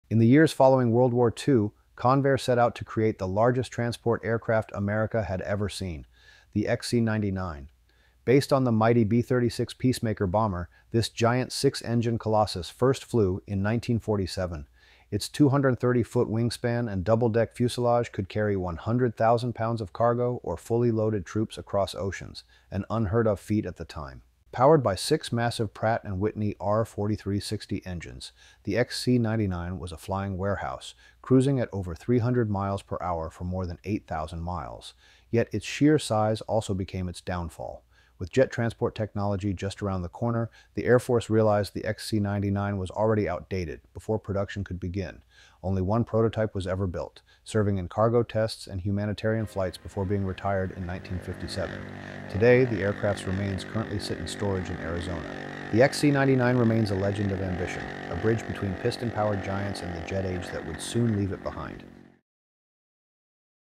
XC-99(Fixed audio)
*Previous version had audio issues at end, still not sure why it got so loud during upload, audio fixed here*
After World War II, Convair set out to create a sky-hauling titan—the XC-99, a massive transport born from the B-36 Peacemaker bomber. With six roaring R-4360 radial engines and a two-deck fuselage, it could lift 100,000 pounds of cargo across continents, making it the largest piston-powered transport aircraft ever built.
But as jet engines arrived, the Air Force’s interest faded. Only one prototype was completed, flying humanitarian and test missions until its retirement in 1957.
Today, the XC-99 stands as a symbol of an era caught between the propeller and the jet—a flying giant that proved size alone couldn’t outpace time.